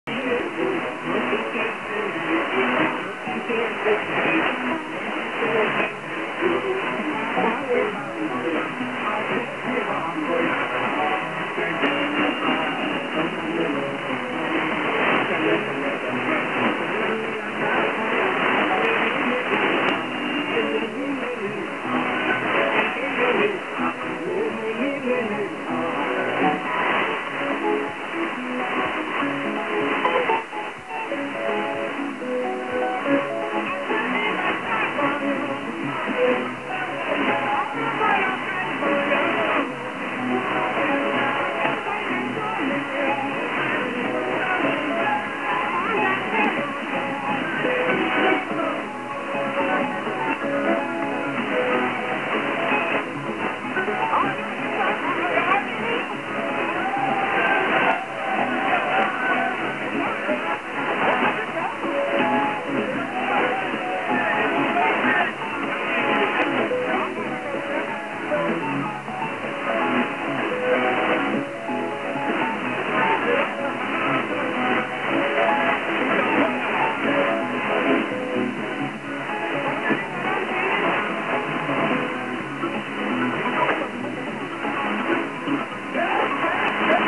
AFRICA No1 on-9580KHz
Received   Sat-04-Dec-2010 22:33(UTC) Saitama JAPAN

Antenna:WSML-PCL